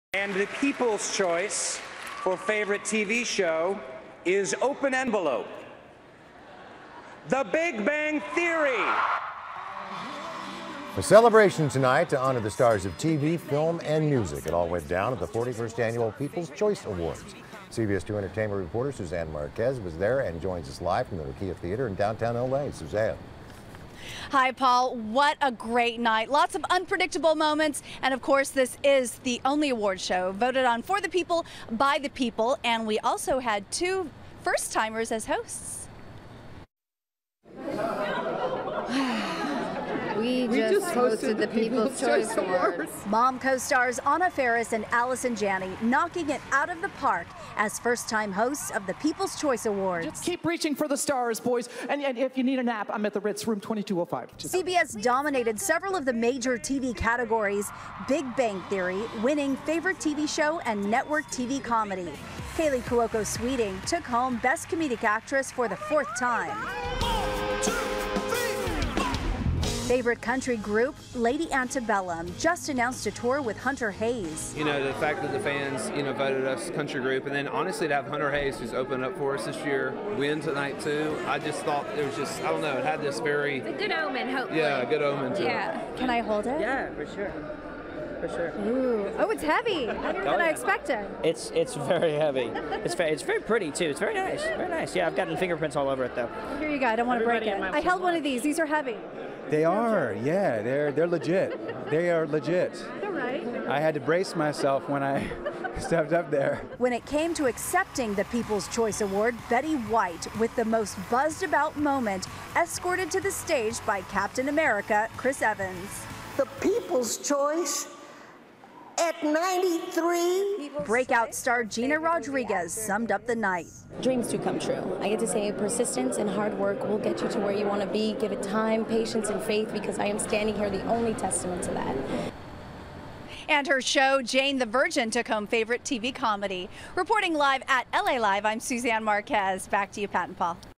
2015 People's Choice Awards Recap
The 2015 People's Choice Awards, with two first-timers hosting, was a success! Suzanne Marques reports.